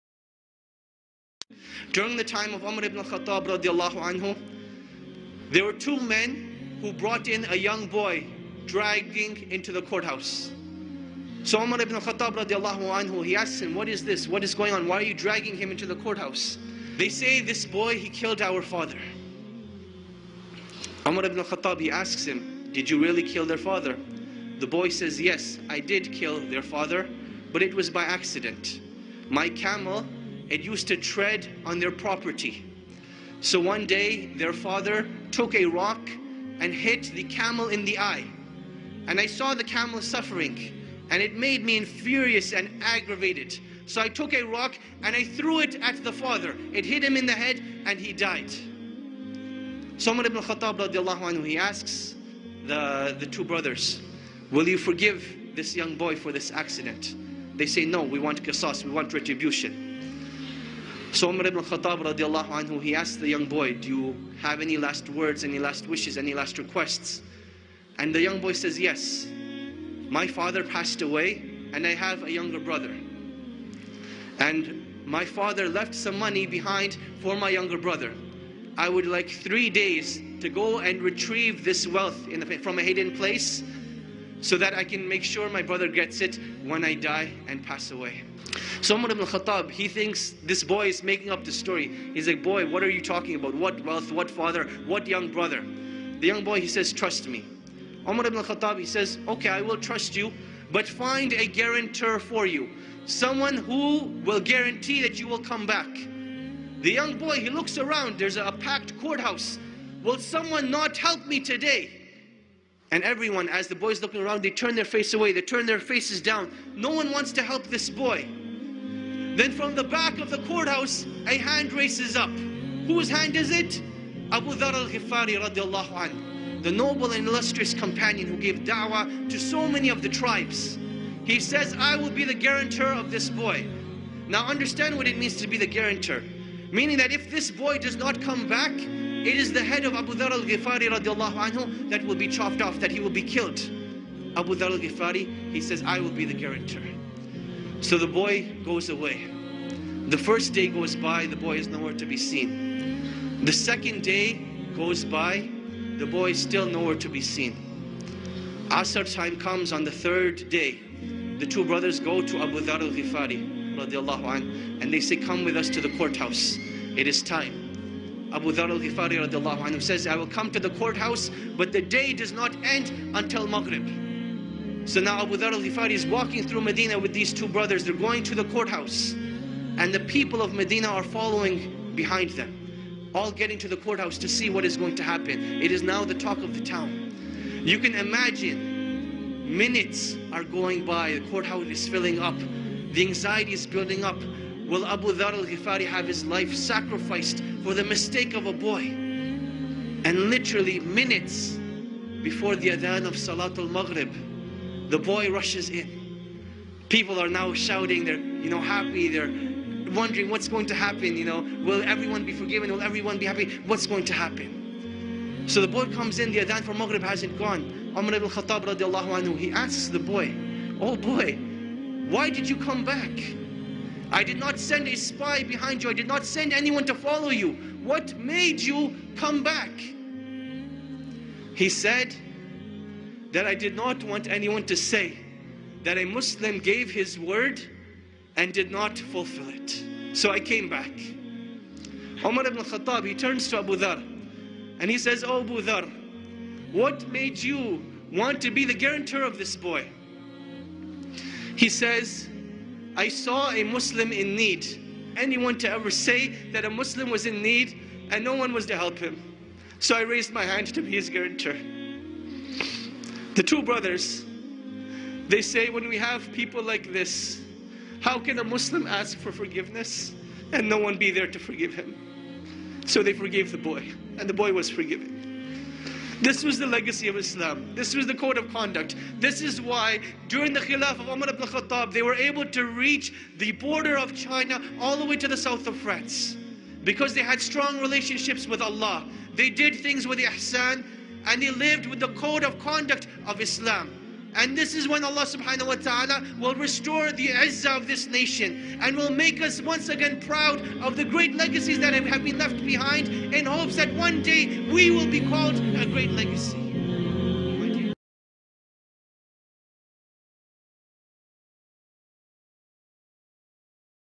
The story a Young Boy And Umar Ibn Khattab (RA) Court
During The Time Of Umar Ibn Khattab (RA)….

There Were Two Men Who Brought In A Young Boy
Dragging Him Into The Courthouse

So Umar (RA) Asked, What Is This? What Is Going On
Why Are You Dragging Him Into The Court House?

They Say, “This Boy Killed Our Father”

Umar (RA) Asked Him
“Did you Really Kill Their Father?”

The Boy Said, “Yes I Kill Their Father
But It Was By Accident”

My Camel It Used To Tread On Their Property

So One Day Their Father Took A Rock
And Hit A Camel In The Eye

And I Saw A Camel Suffering And It Made
Me Furious And Aggravated

So I Took A Rock And I Threw It At The Father
It Hit Him In The Head And He Died

So Umar (RA) HE Asks The Two Brothers
“Will You Forgive This Young Boy For This Accidents”

They Said, “No, We Want Retribution”

So Umar (RA) Asked The Young Boy
“Do You Have Any Last Wards Or Requests?”

And The Young Boy Said, “Yes My Father Passed
Away And I Have A Younger Brother…”

And My Father Left Some Money Behind
For My Younger Brother

I Would Like Three Day To Go And Retrieve
This Wealth From A Hidden Place

So That I Can Make Sure My Brother Gets It
When I Die And Pass Away

So Omar (RA) Thinks This Boy
It Making Up The Story

What Are You Talking About? What Wealth?
What Father? What Young Brother?
The Young Boy Said, “Trust Me”
Umar (RA) Said, “Ok I Will Trust You”

But Find A Guarantor For You, Some One Who Will
Guarantor That You Will Come Back

The Young Boy  He Looks Around
There is A Packed Court House

Will Someone Not Help Me Today?
And Everyone As The Boy Looking Around

They Turn Their Face Away
They Turn Their Face Down

No One Wants To Help Me This Boy

Then From The Back Of The Court House
A Hand Raised Up, Whose Hand Is It?

Abu Dharr Al-Ghifari (RA)

The Noble And illustrious Companion Who
Gave Dawah To So Many Tribes

He Says, “I Will Be The Guarantor Of This Boy”

Now Understand What It Means
To Be A Guarantor

Meaning That If This Boy Does
Not Come Back

It Is The Head Of Abu Dharr Al-Gifari (RA)
That Would Be Chopped Off And Killed

Abu Dharr Al-Gifari He Says, I Will Be Guarantor
So The Boy Goes Away

The First Day Goes By The Boy Is
Nowhere To Be Seen

The Second Day Goes By The Boy Is
Still Nowhere To Be Seen

Asr Time Come Of The Third Day

The Two Brothers Go To Abu Dharr Al-Gifari (RA)

They Said Come With Us To The
Court House, It Is Time

Abu Dharr Al-Gifari (RA) Says,
“I Will Come To The Court House…”

But The Day Does Not End Until Maghrib

So Now Abu Dharr Al-Gifari (RA) Is Walking
Through Madina With These Two Brothers

Going To The Courthouse
And The People Of Madina Following Behind Them

All Getting To The Courthouse To See
What Is Going To Happen

It Is Now The Talk Of The Town

You Can Imaging, Minutes Are Going By
The Court House Is Filling Up

The Anxiety Is Building Up

Will Abu Dharr Al-Gifari (RA) Have His Life
Sacrificed For The Mistake Of A Boy

And Literally Minutes Before The
Azhaan Of Salaah Of Maghrib

The Boy Rushes In, People Are Now
Shouting They Are Happy

Wondering What’s Going To Happen
Will Everyone Be Following

What’s Going Happen

So The Boy Comes In, The Azhaan
For Maghrib Hasn’t Gone

Umar Ibn Khattab (RA) He Askes The Boy
“O Boy Why Did You Come Back?”

I Did Not Send A Spy Behind You
I Did Not Send Anyone To Follow You

What Made You Come Back?

He Said, “I Did Not Want Anyone To Say,
That A Muslim Gave His Word

And Did Not Fulfil It
So I Come Back

Umar Ibn Khattab (RA) Turns To Abu Dharr…

He Says, “O Abu Dharr! What Made You Want
To Be The Guarantor Of This Boy

He Says, “I Saw A Muslim In Need
And I Did Not Want Anyone Ever To Say.

That A Muslim Was In Need And No One
Was There To Help Him

So I Raised My Hand To Be His Guarantor
The Two Brothers They Say,
“When We Have People Like This”

How Can The Muslim Ask For Forgiveness?
And No One Be There To Forgive Him

So They Forgave The Boy
And The Boy Was Forgiven

This Was The Legacy Of Islam
This Was The Code Of Conduct

This Is Why During The Caliphate Of
Umar Ibn Khattab (RA)

They Were Able To Reach The Border Of China
All The Way To South Of France

Because They Have Strong Relationship With ALLAH
The Did Things With Ihsaan

And They Lived With The  Code Of Conduct Of Islam

And This Is When ALLAH (SWT) Will Restore
The Honour  Of This Nation

And Will Make Us Once Again Proud Of The
Great Legacies That Have Been Left Behind

In Hope That One Day, We Will Be
Called A Great Legacy